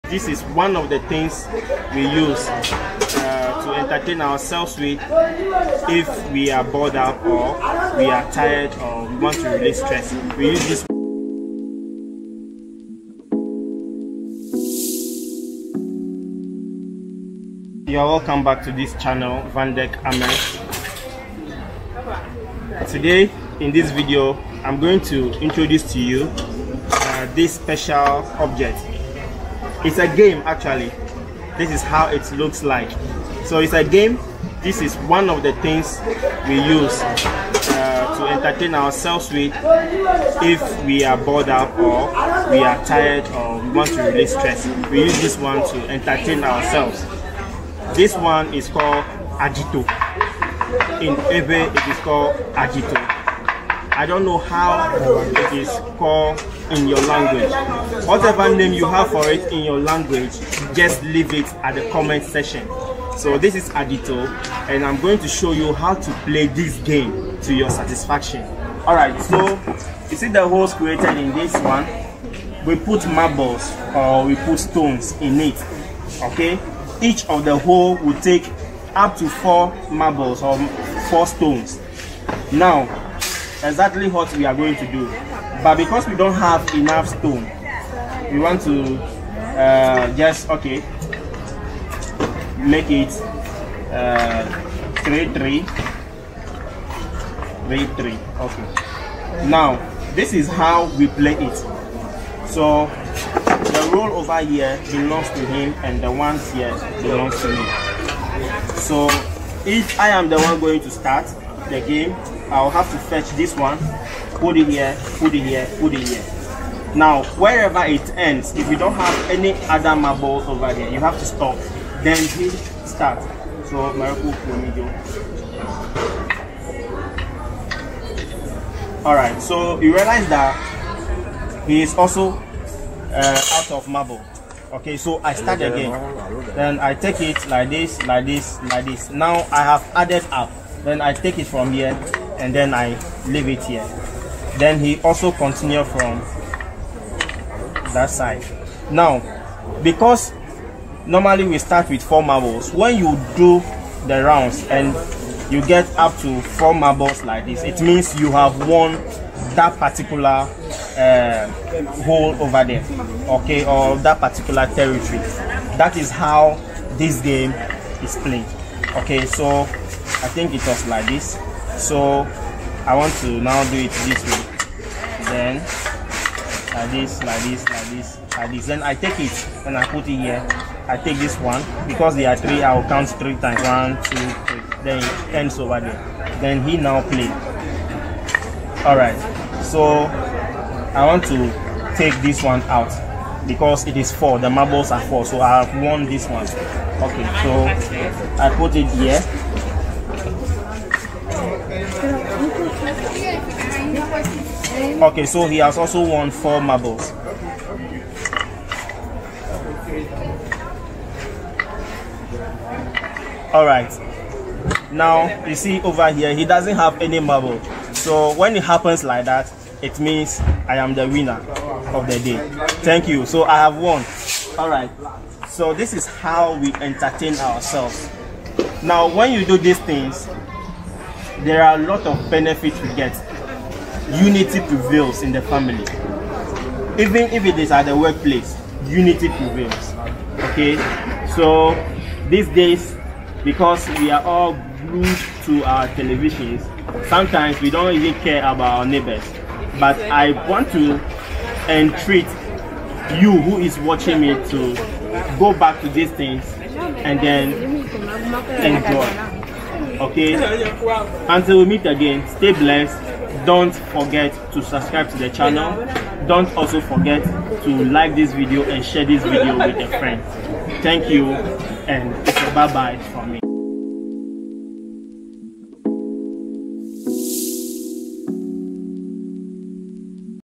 Adito (Also Known as Oware)
This video is about Adito, also known as Oware, one of the oldest and most beloved traditional board games in Africa. Adito, often referred to as the "Game of Wisdom," has been played for centuries and is deeply rooted in the cultural heritage of many African societies.

Whether you are a seasoned player or new to Adito, this video provides an in-depth look at the game, its variations, and its role in promoting strategic thinking and social interaction. Join us on this journey to discover why Adito remains a cherished pastime and an enduring symbol of African ingenuity and tradition.